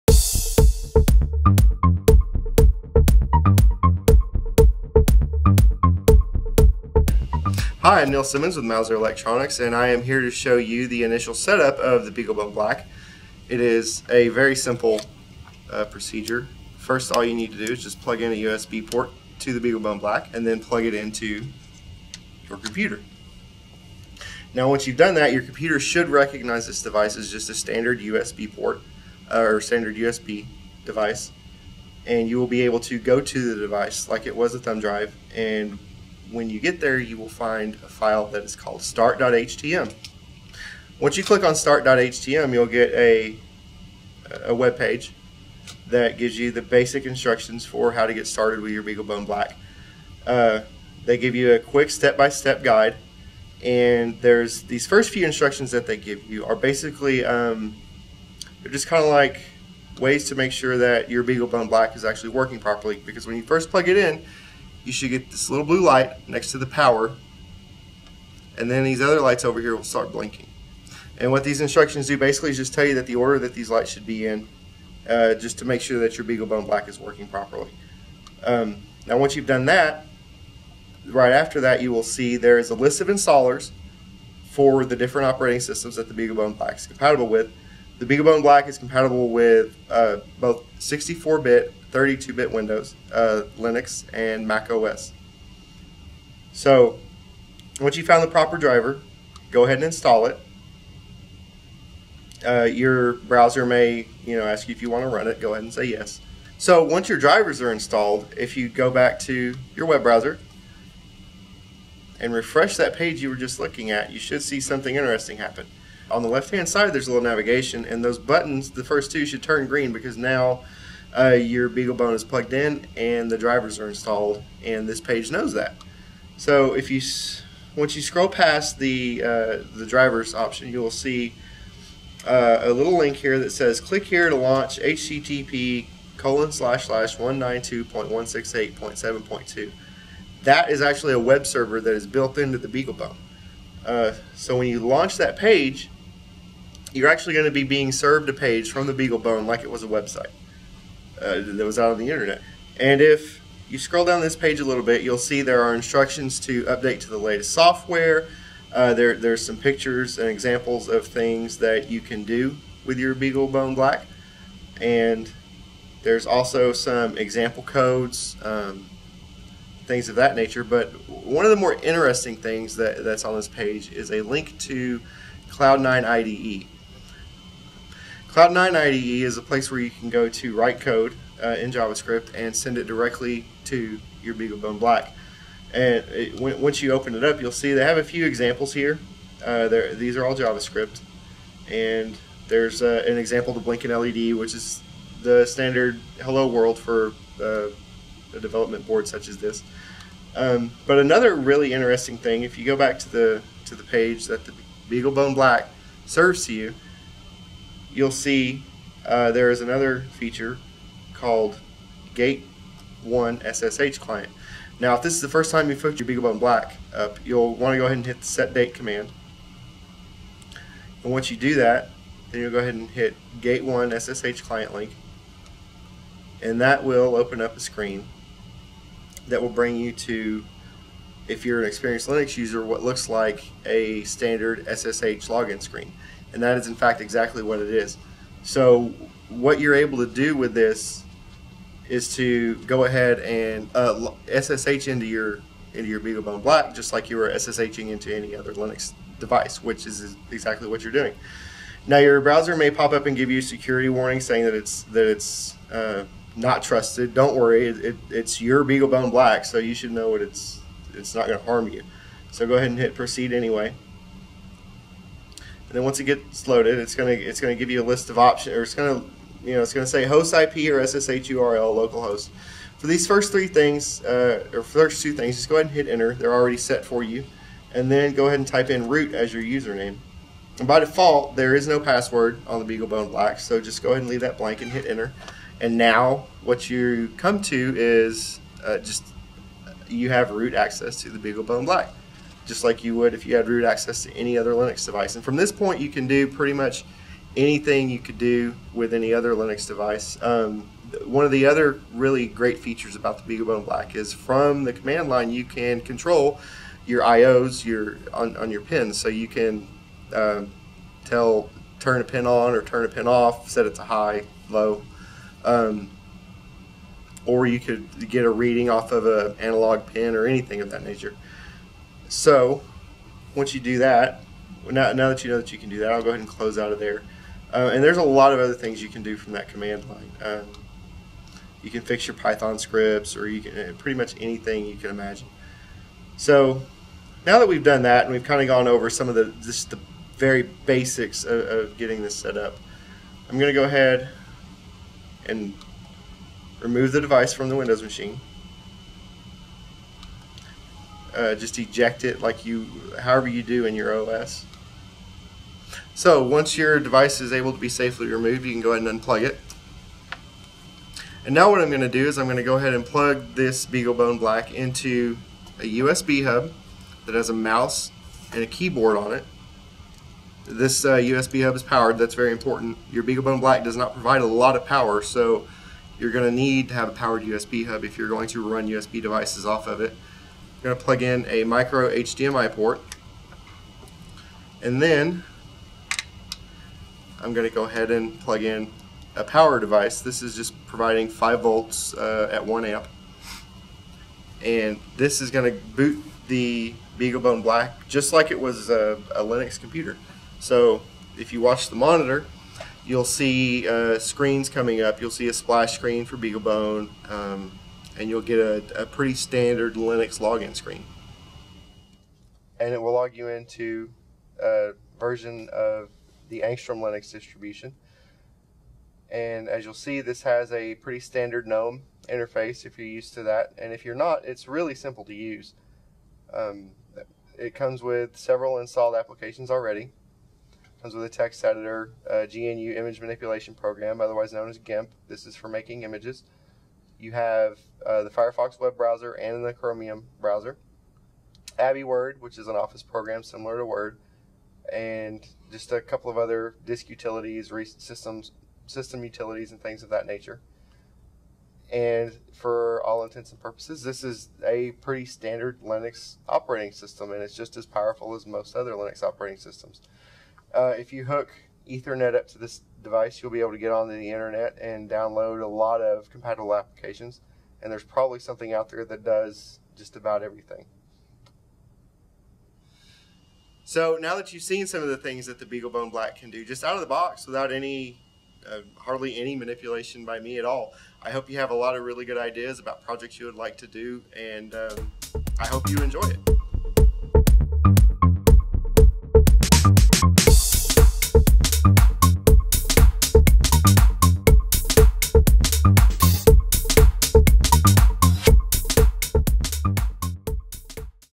Mouser Electronics -- First-Time Setup of the BeagleBone Black
We take you through initial set-up of the BeagleBone Black, beagleboard.org's NEW high-power and low-cost development platform!